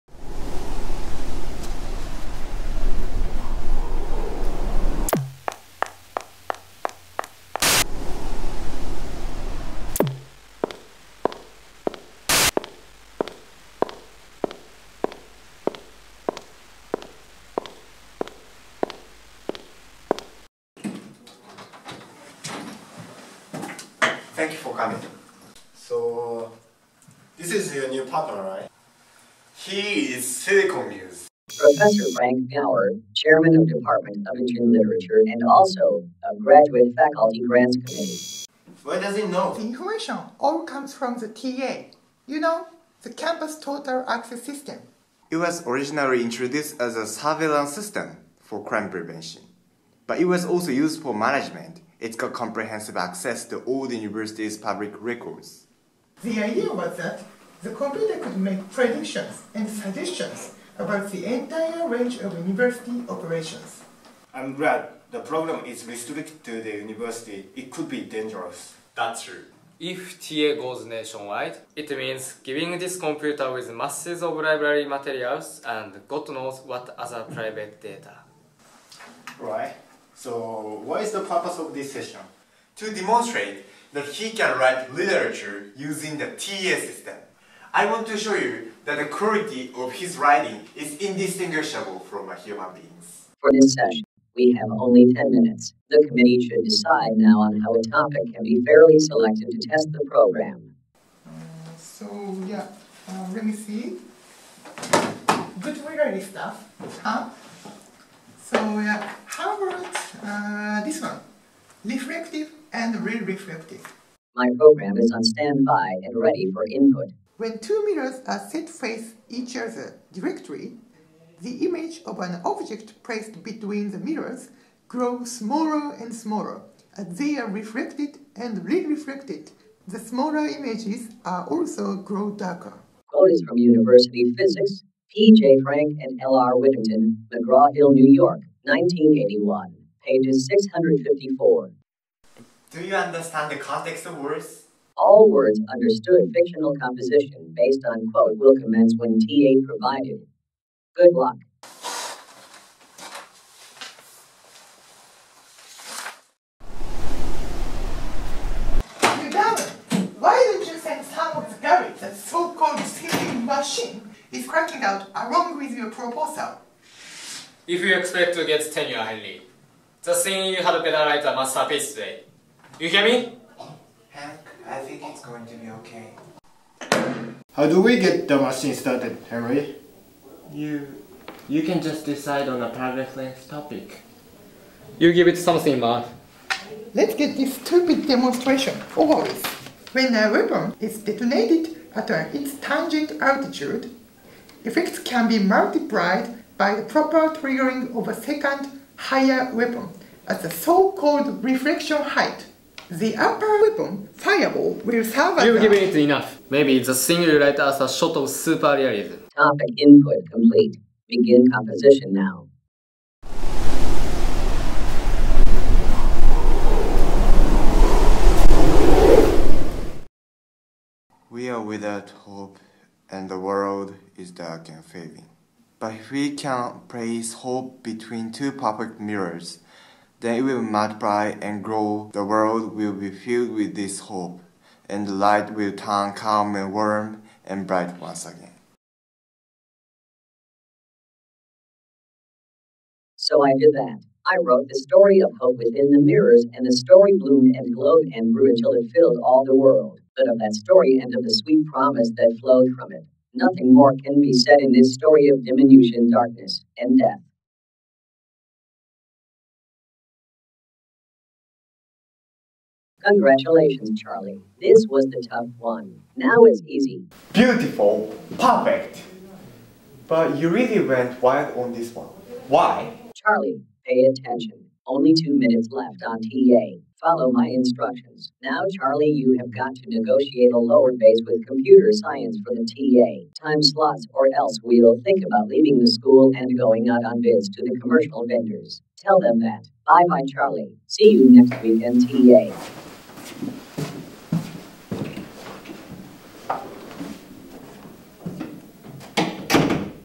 GEH024 YAMAZAKI Jo Final Project Silicon Muse
This student video is an adaptation of the Herbert Schenck short story, 'Silicon Muse' (1984).